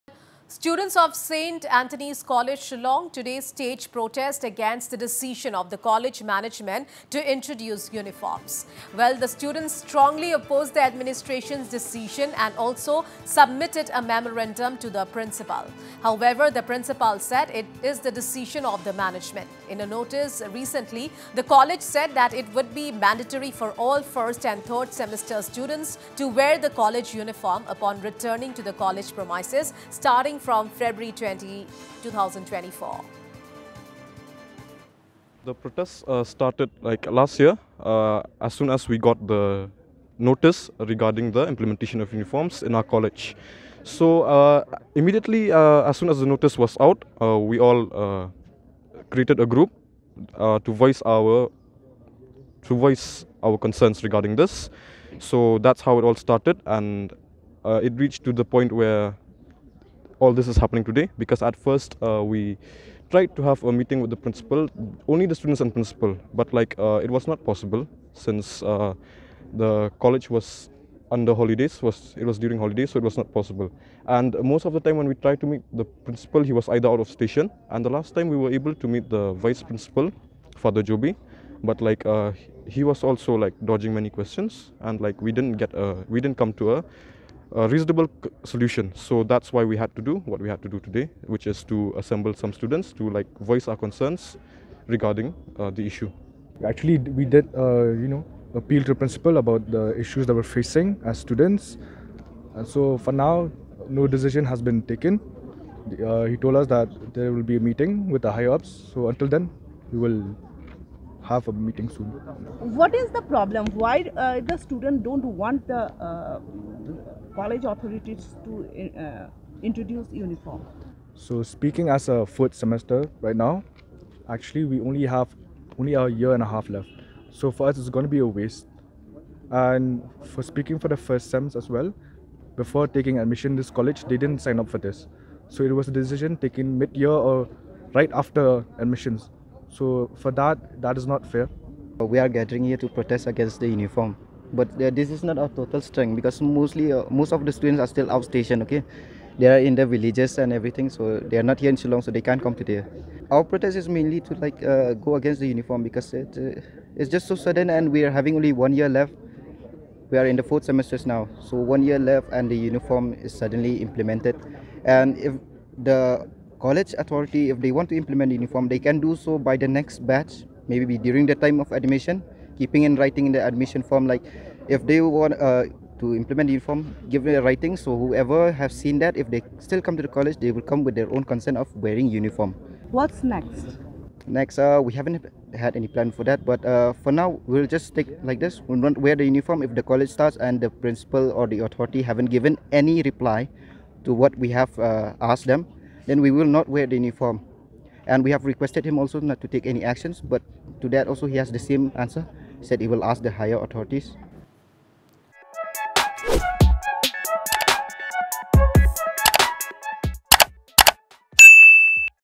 Meghalaya: St. Anthony’s College students protest introduction of uniforms
🟦🟥 𝗡𝗢𝗥𝗧𝗛𝗘𝗔𝗦𝗧 𝗟𝗜𝗩𝗘 | 𝗠𝗘𝗚𝗛𝗔𝗟𝗔𝗬𝗔

Students of St Anthony's college, Shillong today staged protest against the decision of the college management to introduce uniforms.